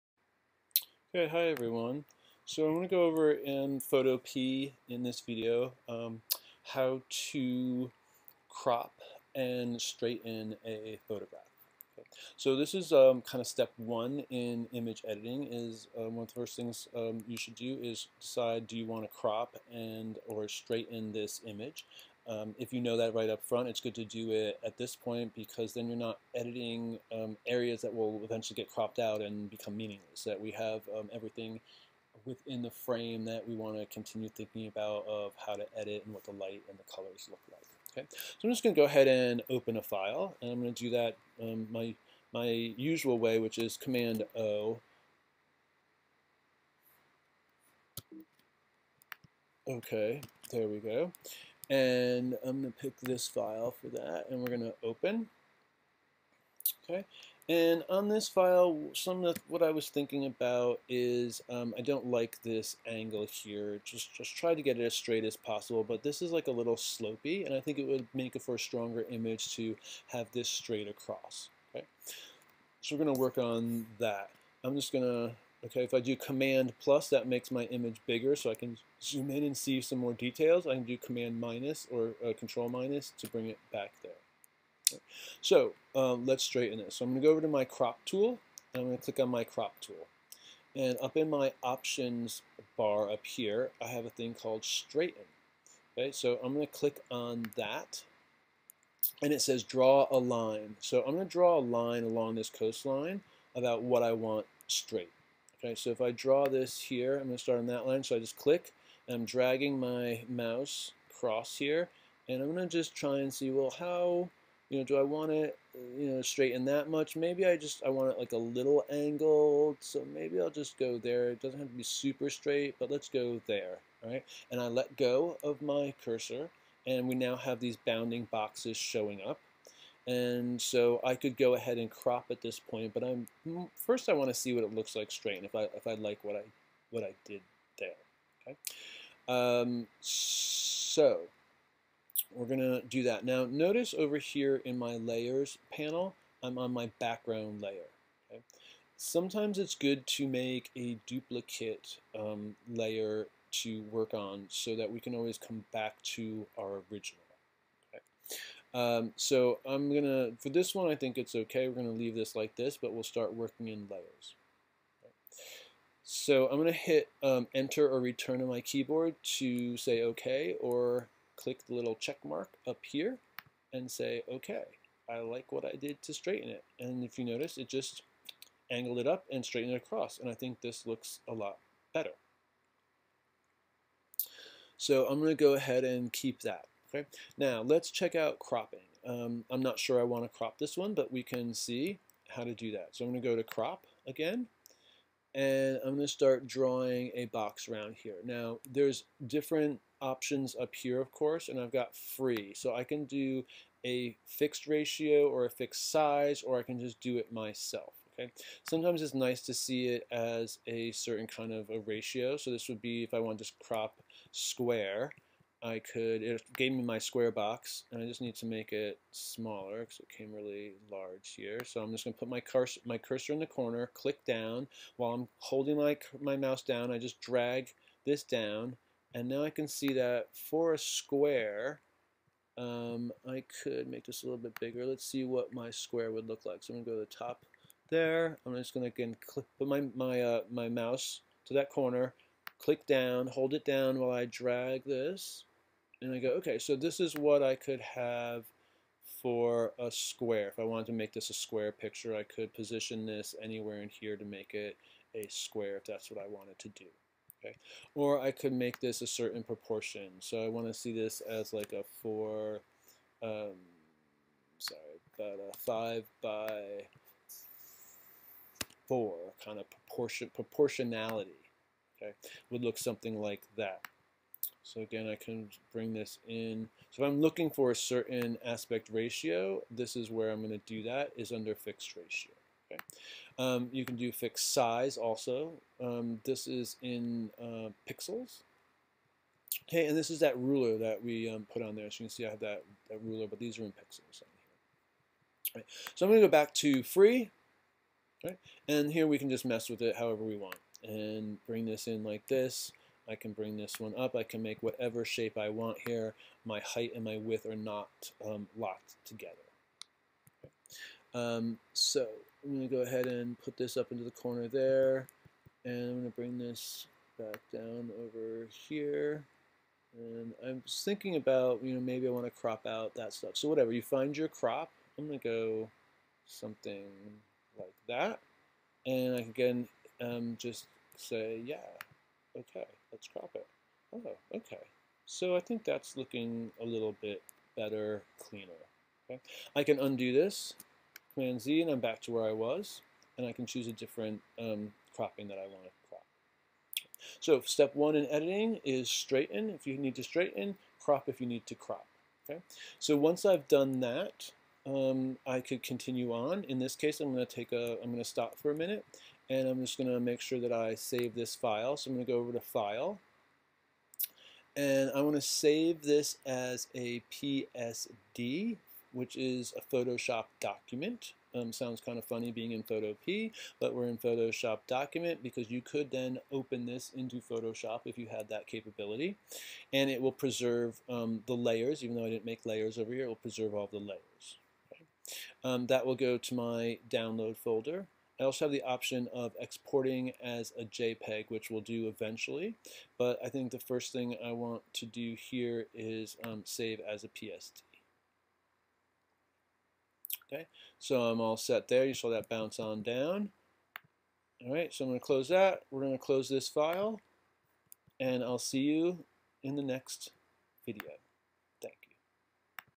Photopea How to Crop and Straighten
This video show you how to use the Crop Tool in Photopea to straighten an image. It shows you how to use the Crop Tool to crop to different proportions.